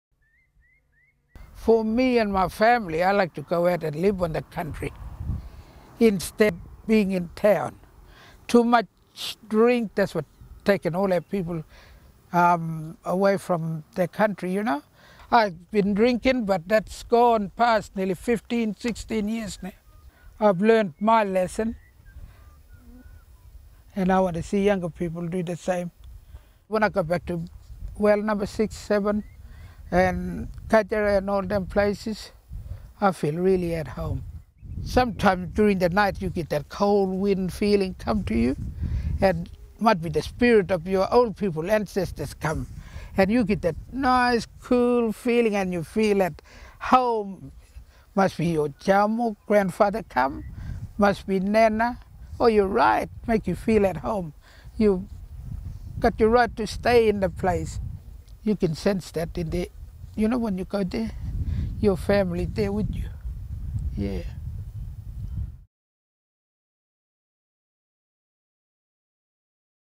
Interplay Project | Lena Long | Health
Martu Ranger Lena Long outlines the significance of being on country with family.

This interview is part of the Interplay Project, a national study on Aboriginal wellbeing in remote Australia.  The Interplay Project is supported by the Australian Government Cooperative Research Centres Program through the Cooperative Research Centre for Remote Economic Participation (CRC-REP). The views expressed herein do not necessarily represent the views of the CRC-REP or its participants.